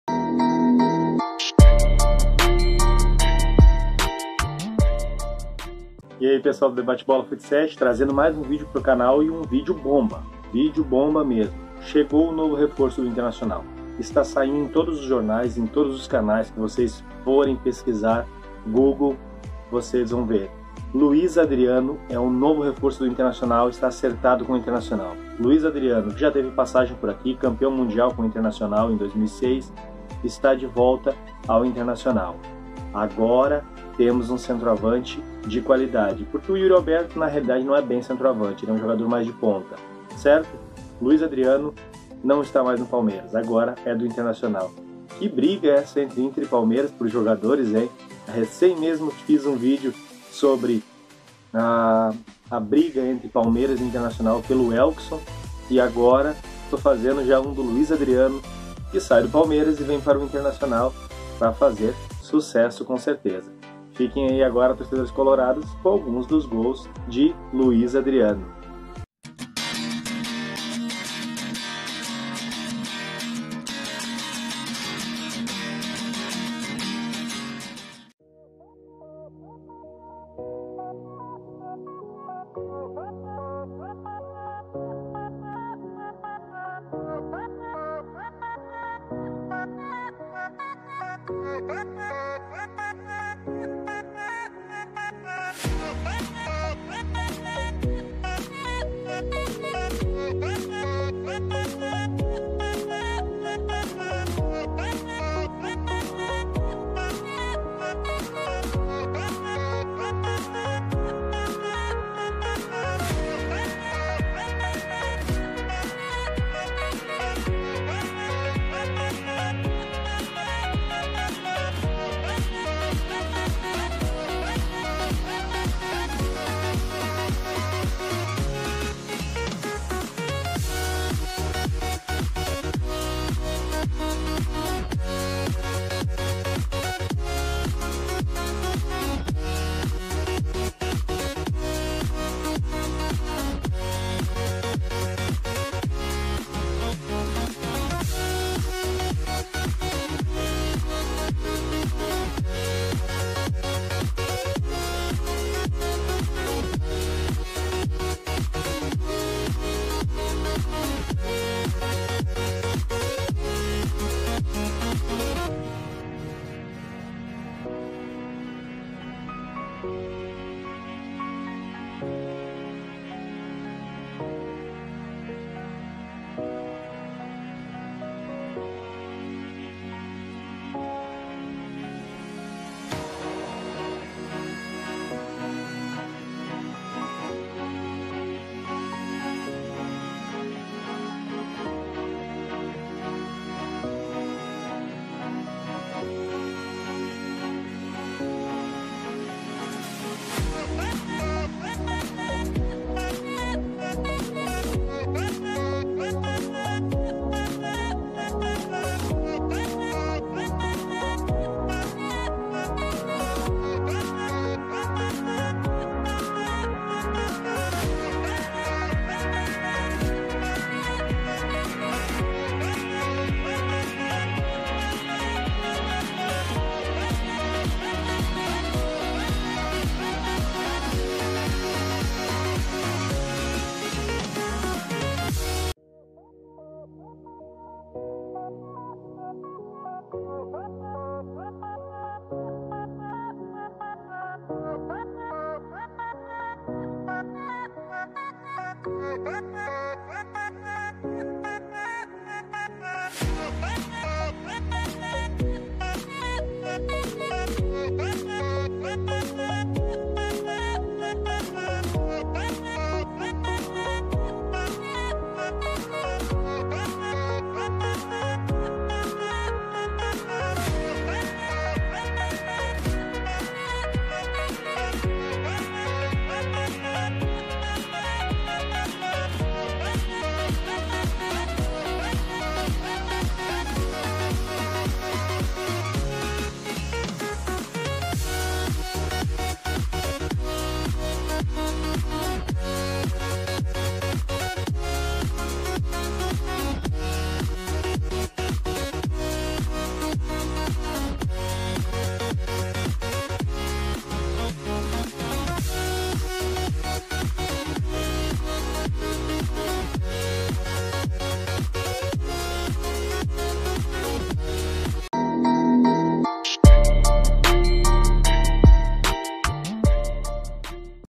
Luiz Adriano volta para o INTERNACIONAL
Luiz Adriano está de volta ao time que o lançou para o mundo da bola.
O INTERNACIONAL vai contar com esse belo reforço na proxima temporada.